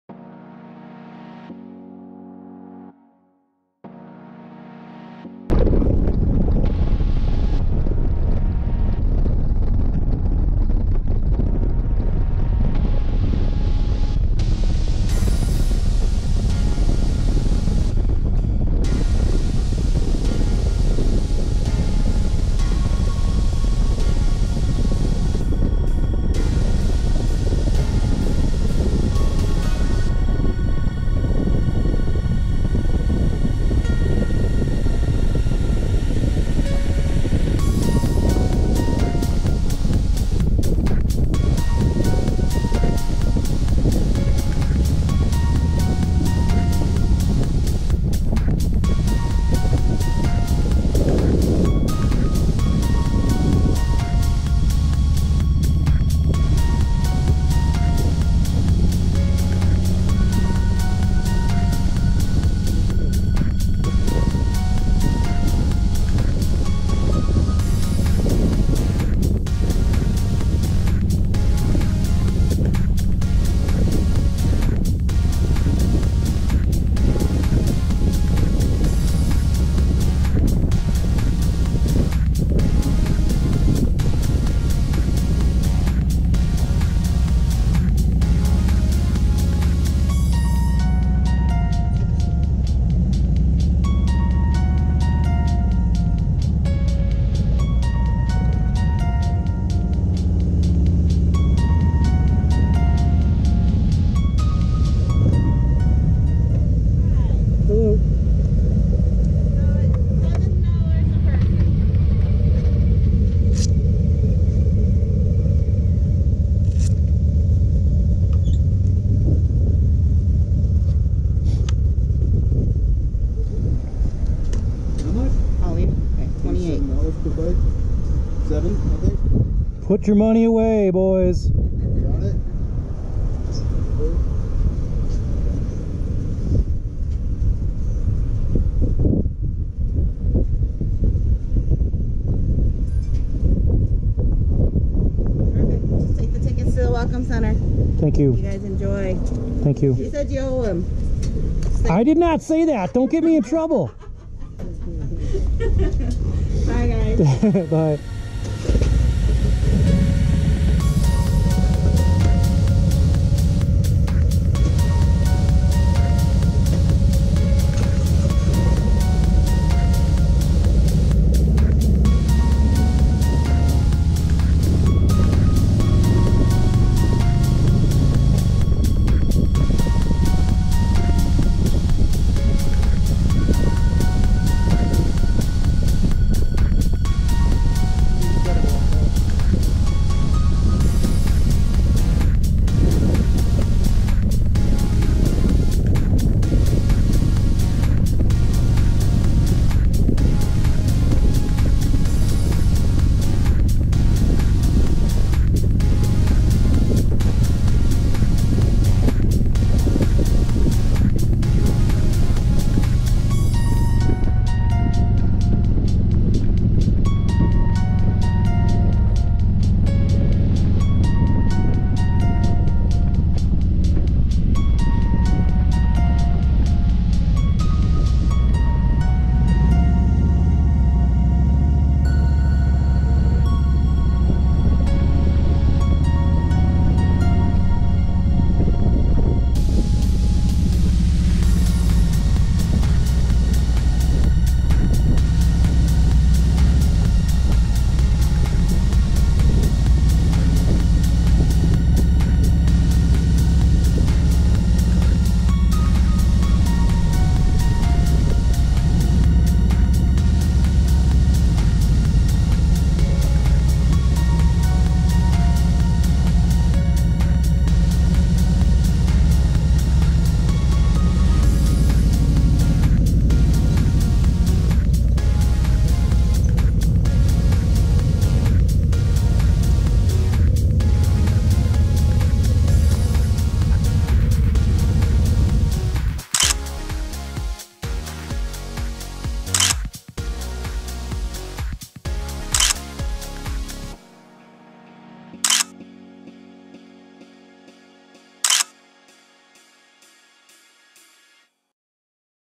Ride up to the Crazy Horse Memorial
Today we take a trip up to the Crazy Horse Memorial and this is the ride up to the site.